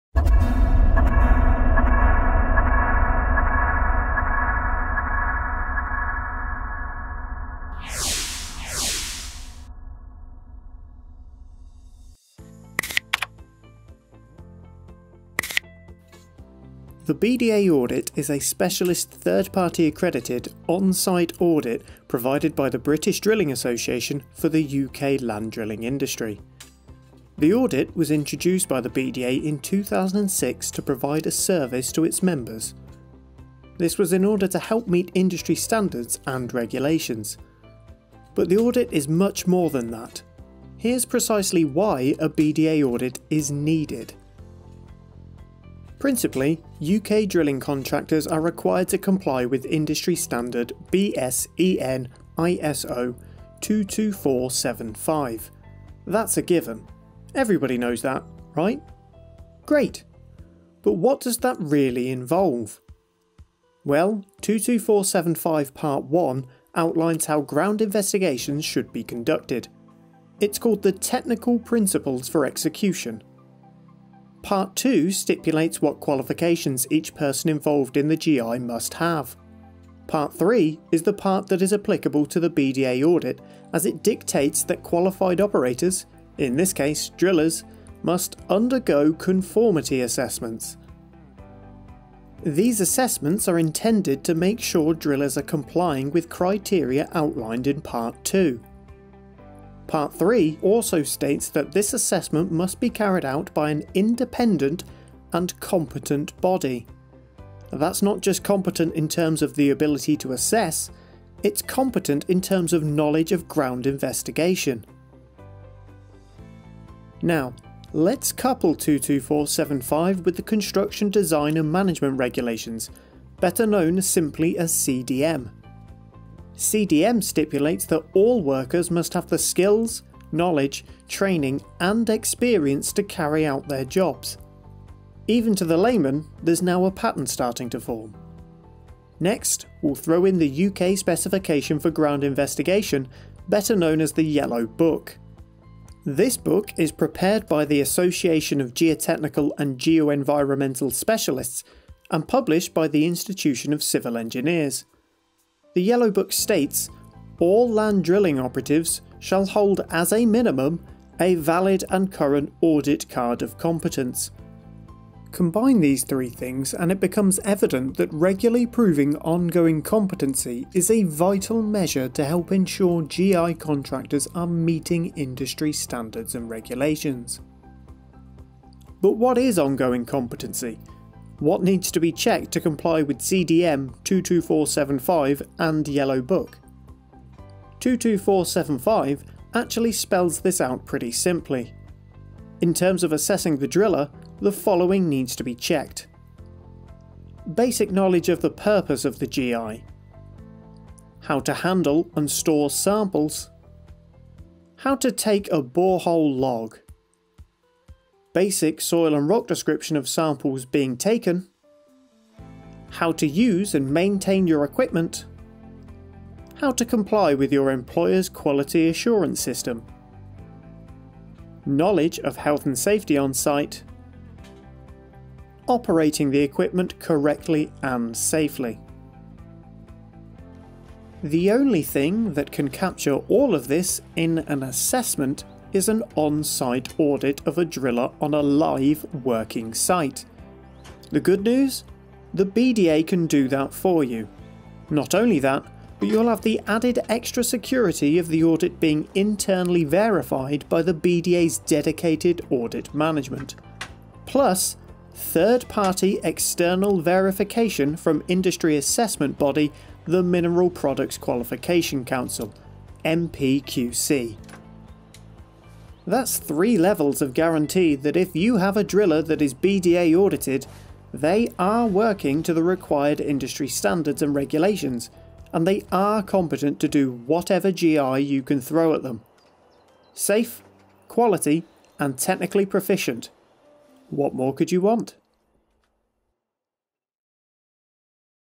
BDA Audit Why
The British Drilling Association (BDA) Audit satisfies British Standards (BS 22475 Part 2 and 3), the UK Specification for Ground Investigation and Construction Design and Management Regulations (CDM), regarding the need to prove the ongoing competence of drilling operatives.

The BDA has many aims and objectives, ranging from improving efficiency, health and safety and standards in the industry, through to more indirect, such as promoting innovation, training, establishing codes of practice and much, much more. All these objectives are bound together by the BDA Audit, and a commitment to be audited is to sign up to all aims and objectives of the BDA.

There is much to celebrate about the BDA Audit; since its inception, it has helped elevate the drilling sector to professional status, in addition to proving a catalyst to drive up standards that the sector so badly needed, it also provides a true differentiator for BDA members, as a tool to use to compete and win business.

Whilst providing only a snapshot of the Audit process, the video shows how simple the Audit procedure is and dispels the myth of any inconvenience associated with it. Any impact caused during the short period of time the Audit takes place, is far outweighed by the benefits that it brings to the operatives and the businesses for which they work.

The BDA Audit will always be an evolving process, but as a differentiator for those committed to improving all aspects of drilling, it is without rival, and with an increasing number of clients looking at proof of competence as part of their pre-qualification process, the BDA Audit is an industry must.